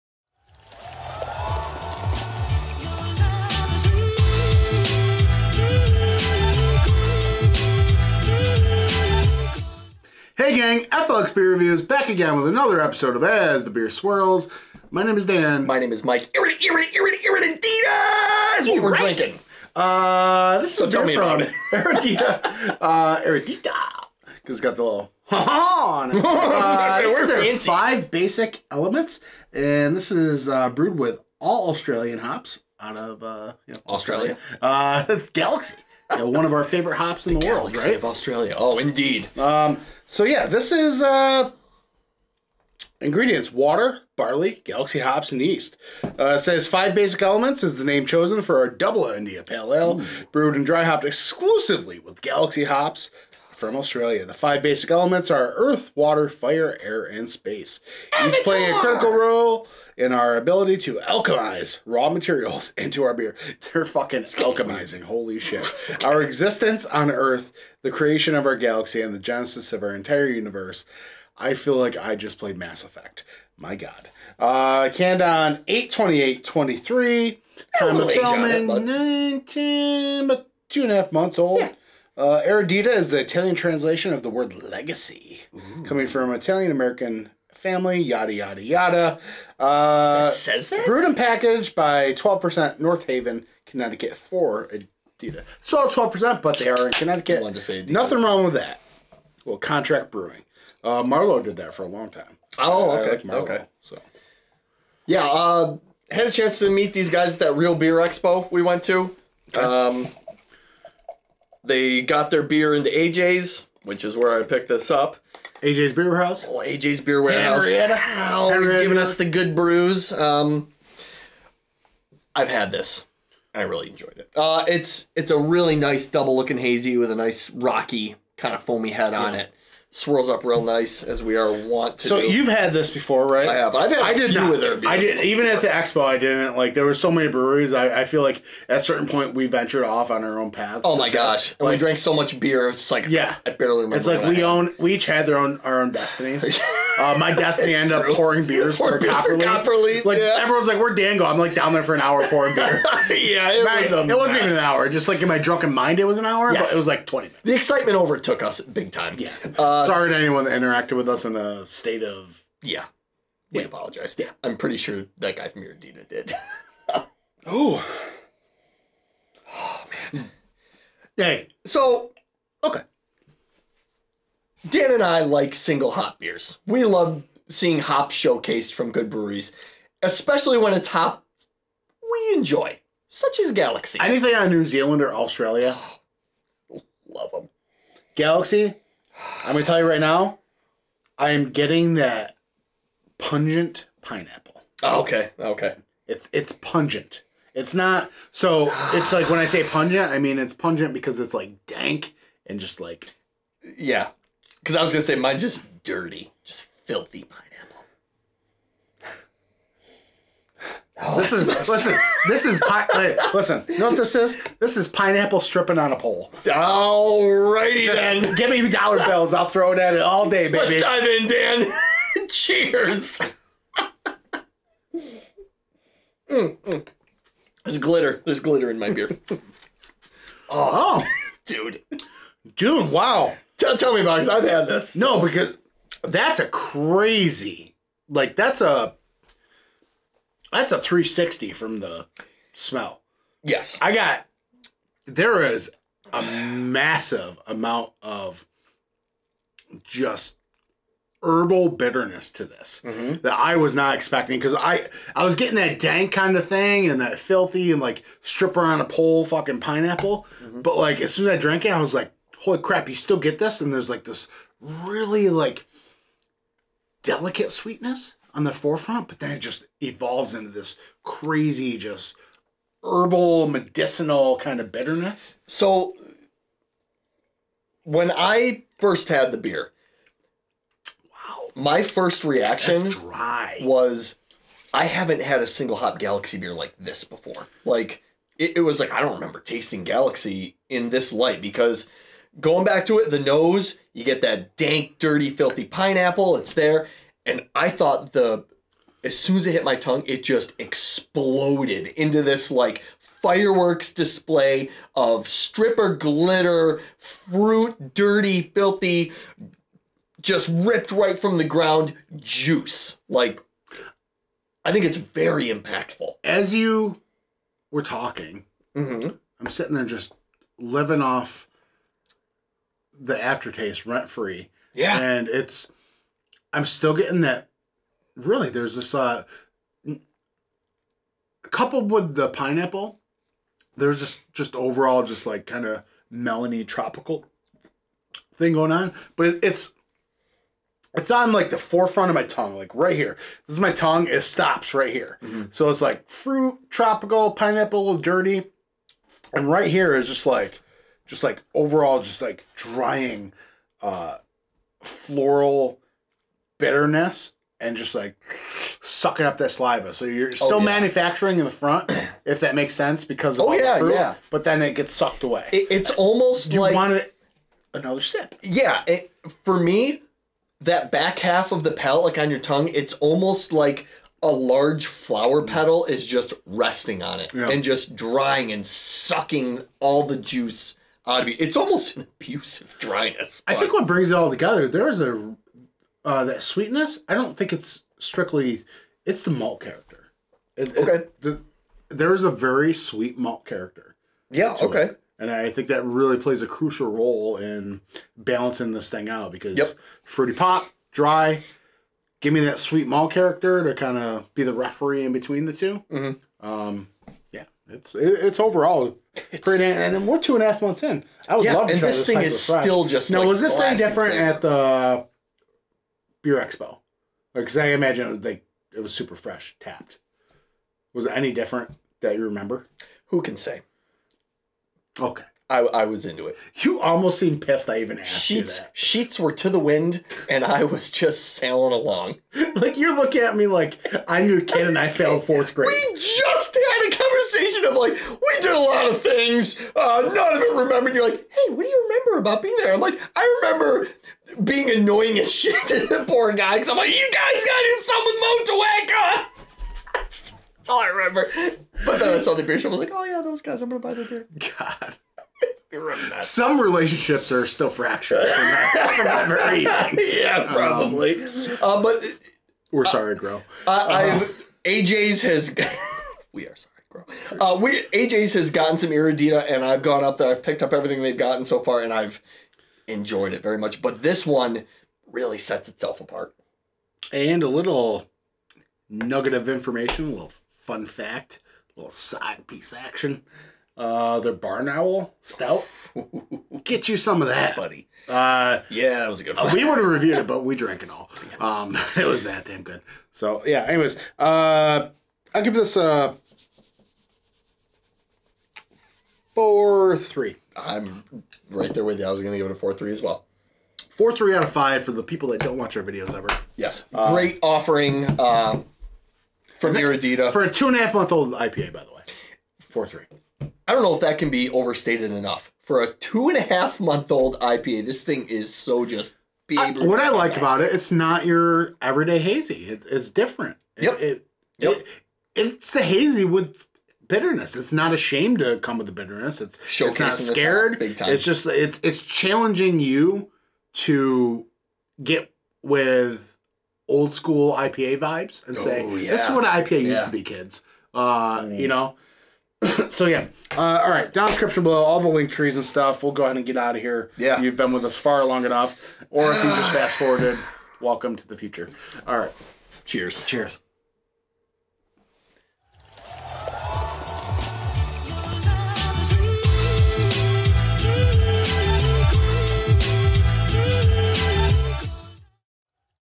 Eredità Beer | Five Basic Elements: Galaxy (Imperial IPA) | Beer Review #484
Hey gang! Today we try a beer from Eredità Beer located in North Haven, CT. This is their Five Basic Elements which is an imperial/double india pale ale and comes in at 8% abv. The description is as follows:

Double IPA brewed and dry hopped entirely with Galaxy hops from Australia.

Notes of tropical fruit, peaches, and citrus accompanied by sturdy bitterness.